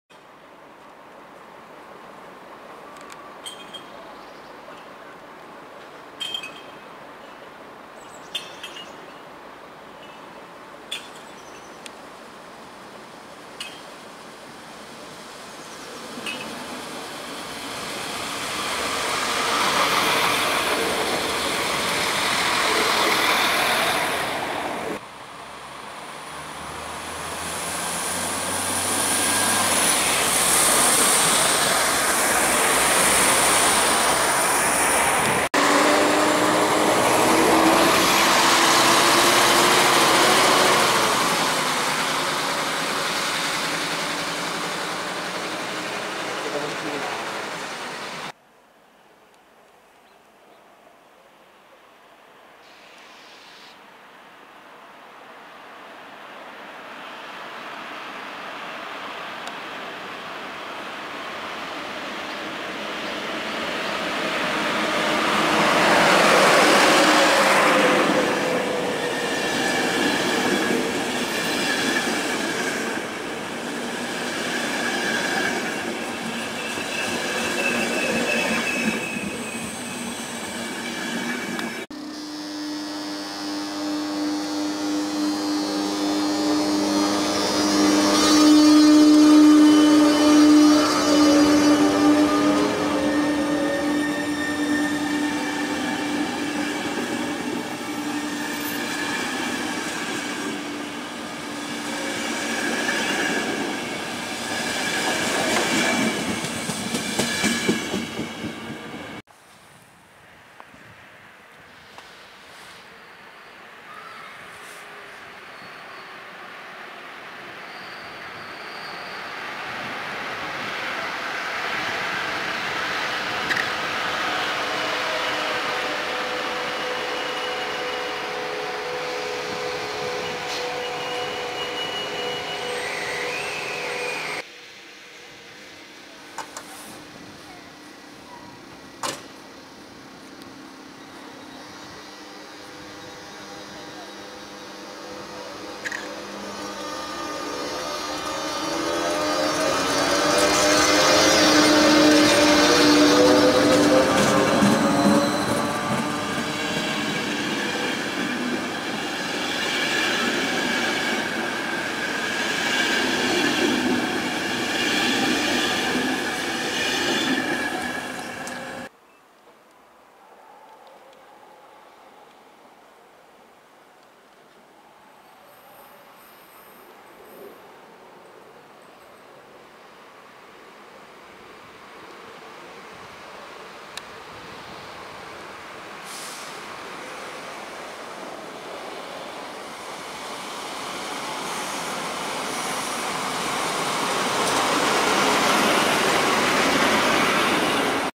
Vlaky - Mariánské Lázně - 27.12.2019
Sestřih příjezdů a odjezdů vlaků ze stanice Mariánské Lázně.